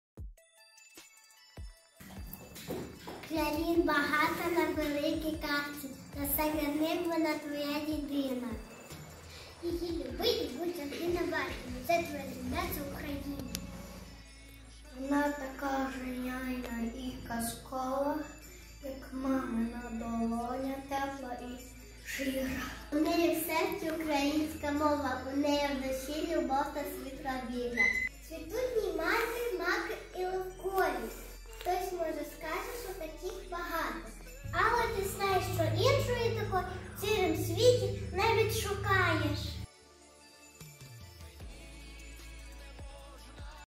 Вірш "Україна" Мар'яни Савко.
Барбір Артем, Герман Каріна, Хапіцька Софія, Шаригіна Катерина, Шкрібляк  Поліна, учні 1-б класу Чернівецької гімназії №16, вихованці гуртка "Стежинками природи" МЦЕНТУМ. Керівник: Пеліховська А.І